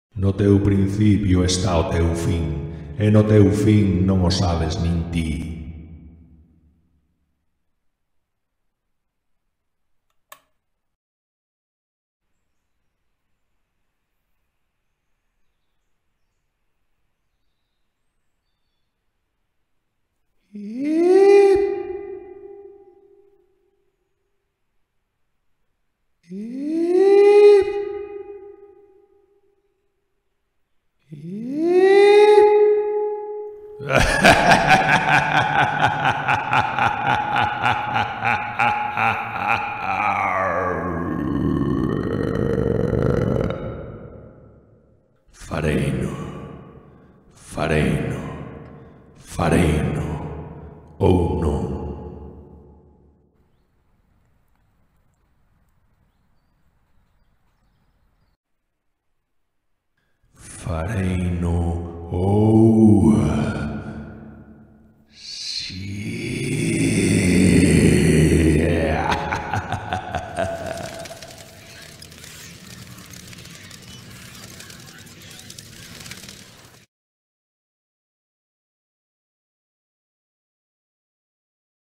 CAGARSE DE MEDO Unha curtametraxe de humor negro.
CAGARSE DE MEDO
Unha curtametraxe amateur de humor negro.
Autor, Xermán Son (2022).